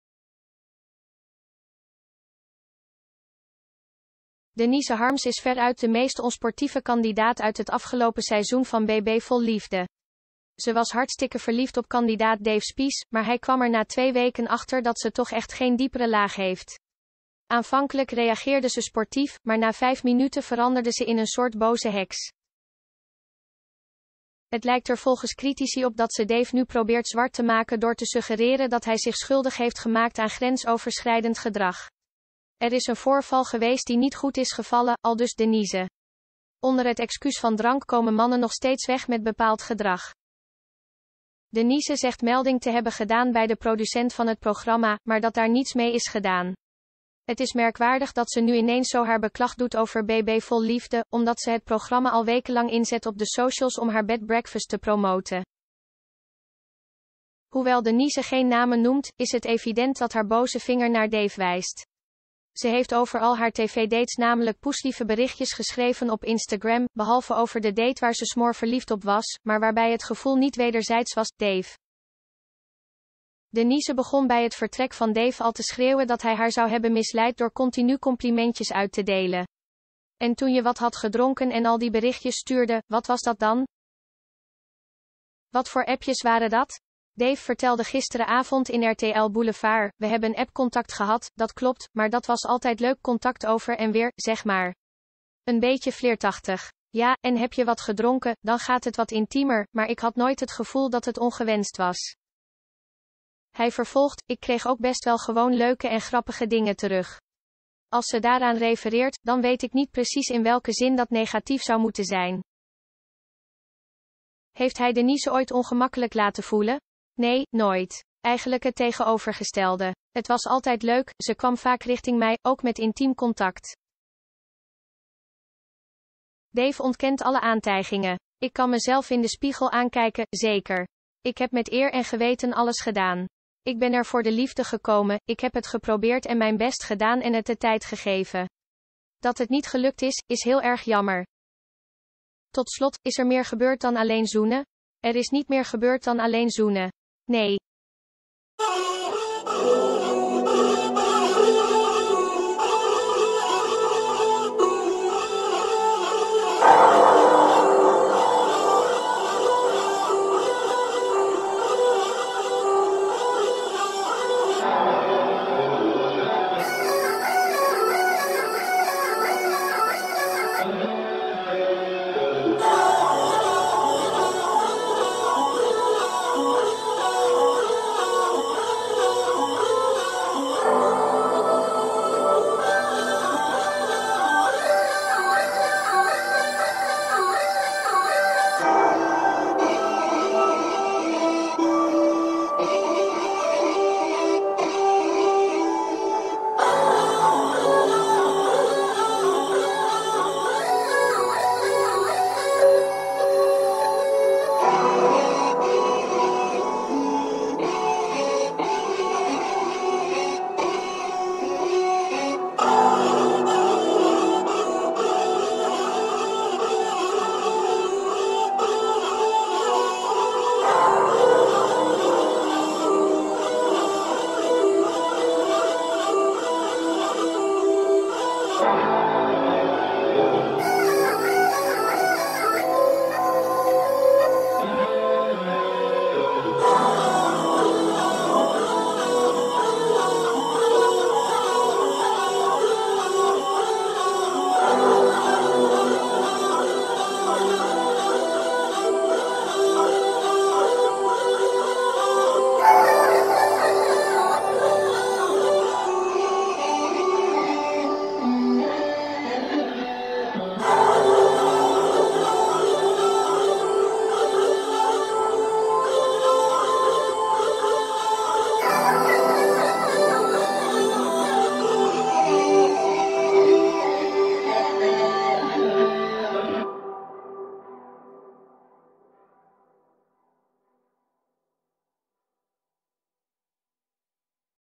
B&B Vol Liefde-ster Dave ontkent grensoverschrijdend gedrag
B&B Vol Liefde-ster Dave Spies ontkent dat hij zich schuldig heeft gemaakt aan grensoverschrijdend gedrag. “Ik kan mezelf in de spiegel aankijken. Ik heb met eer en geweten alles gedaan.”